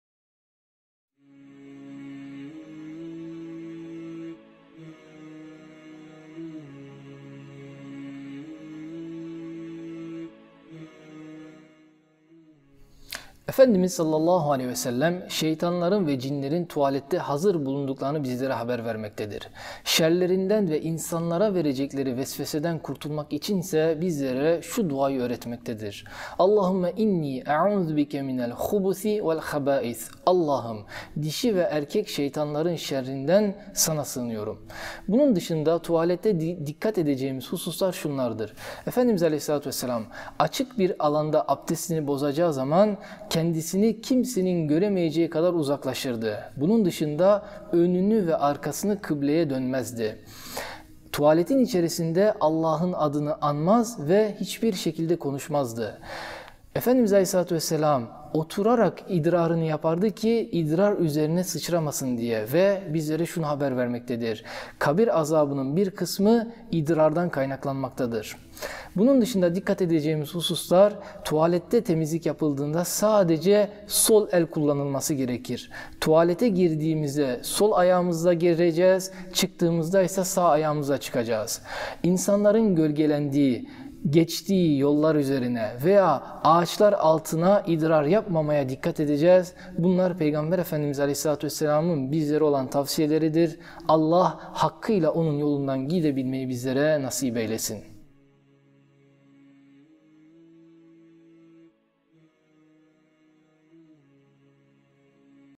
Ertuğrul Yılmaz : Tuvalette Güvende Miyiz ? (5. Bölüm)
Her alanda bizlere doğru edebi öğreten sevgili Peygamberimiz (sav), tuvalete girme âdâbını da öğretmektedir.

Helaya nasıl girmemiz gerektiğini, nelere dikkat etmemiz gerektiğini öğrenme vakti.